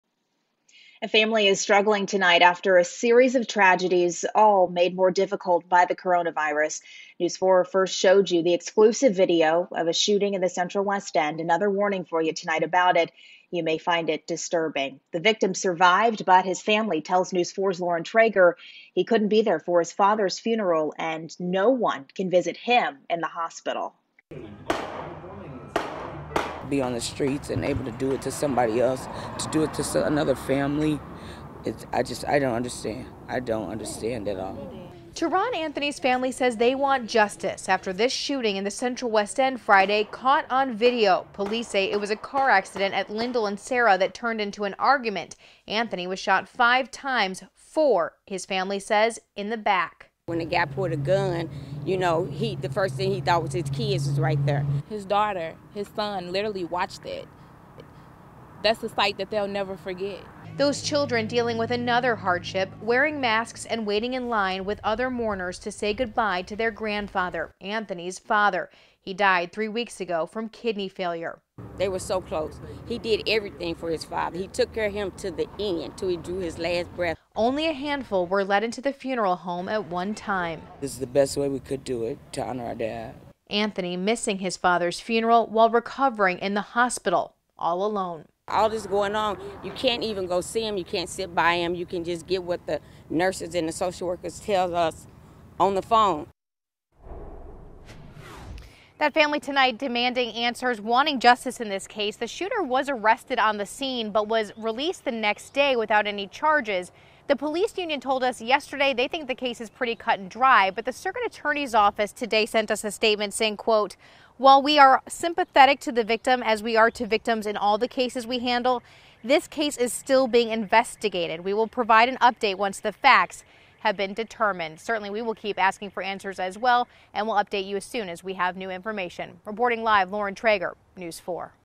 Family of shooting victim demands justice as virus outbreak keeps them away from him
A series of tragedies has hit a local family and the coronavirus outbreak has made it even more difficult, forcing them to be apart.